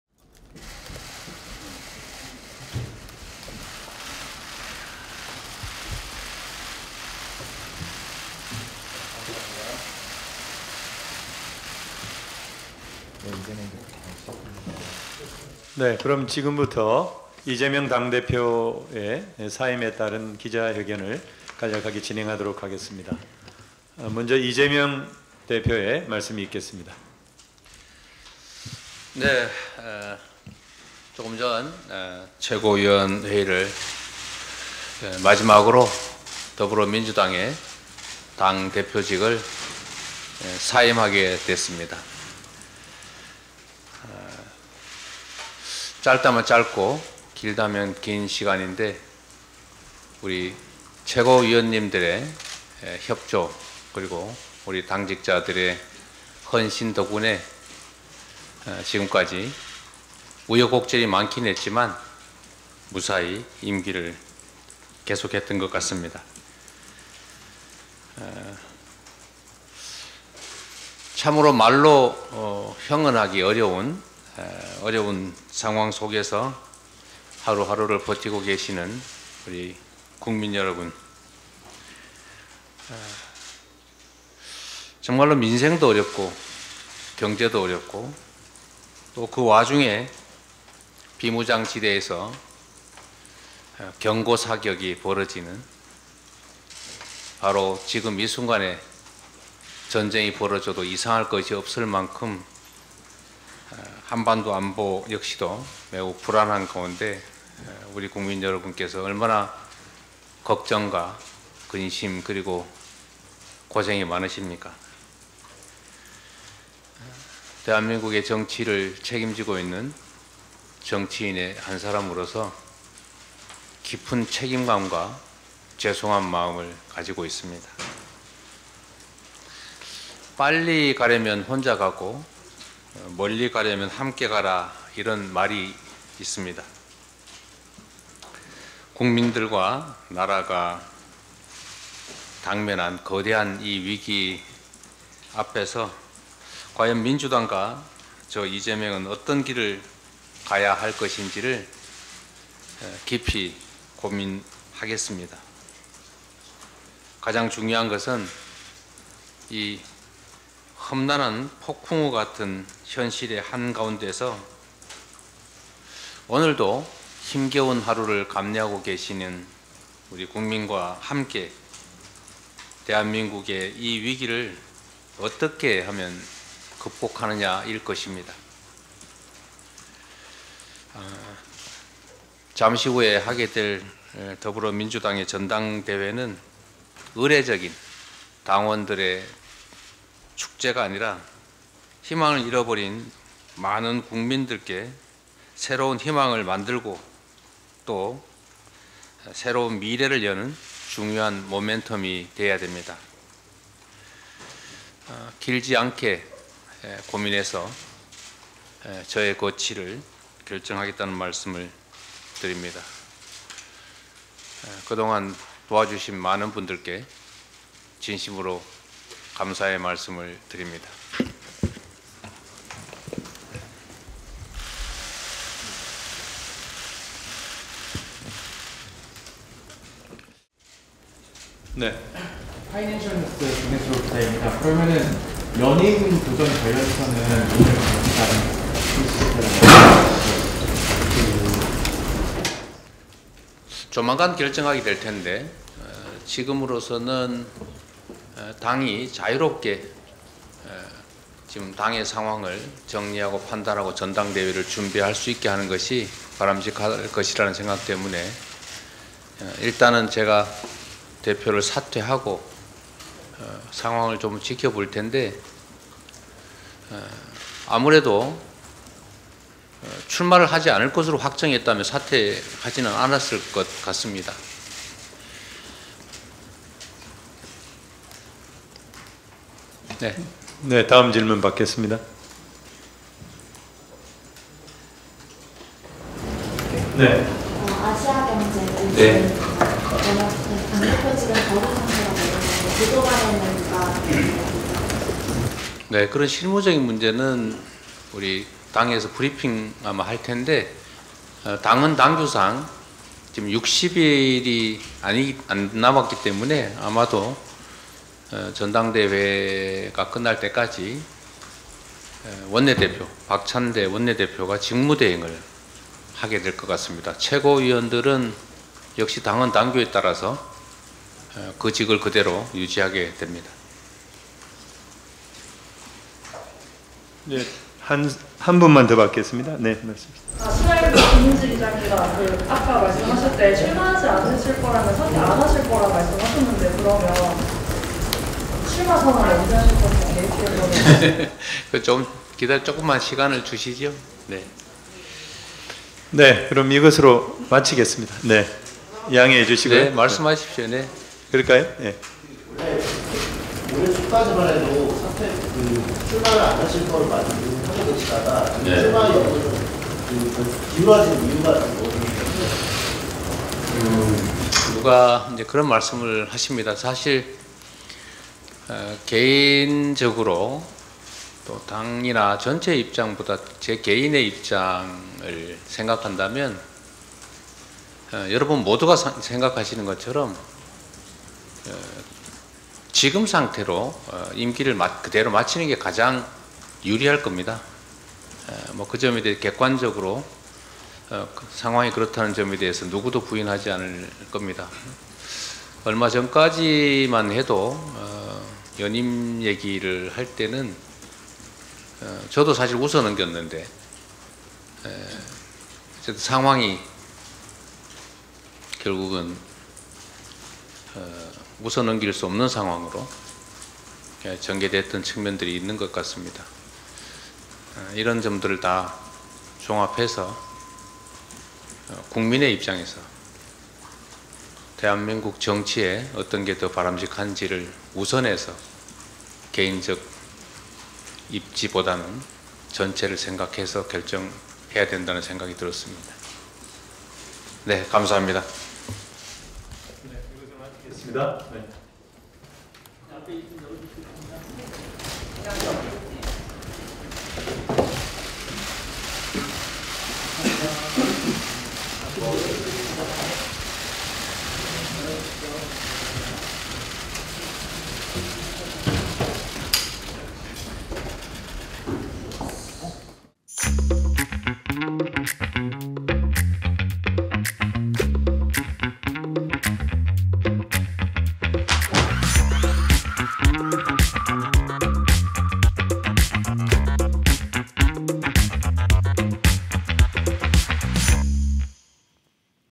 [LIVE] 이재명 민주당 대표, '연임' 수순
이재명 더불어민주당 대표가 연임을 위해 오늘(24일) 대표직에서 물러나기로 했습니다.
이 대표는 자신의 거취에 대해 조만간 입장을 밝히겠다고 했습니다.
다만 차기 당 대표 선거 불출마를 확정했다면 사퇴하지 않았을 것이라며 연임 도전을 시사했습니다.
오는 8월 열리는 민주당 전당대회까지 이 대표의 직무대행은 박찬대 원내대표가 맡습니다.

제작 : 뉴스토마토

감사합니다.